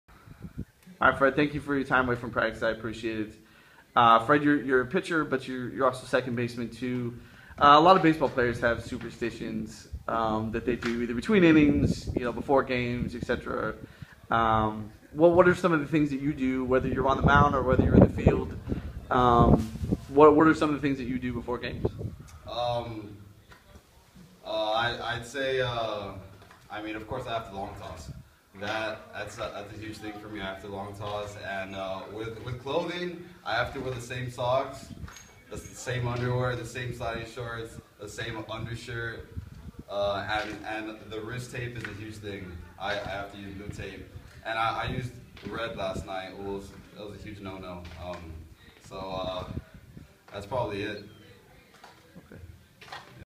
Brownsville Veterans Memorial's Fred Villarreal
Brownsville Veterans pitcher/second baseman Fred Villarreal talks about his supersitions before playing a baseball game.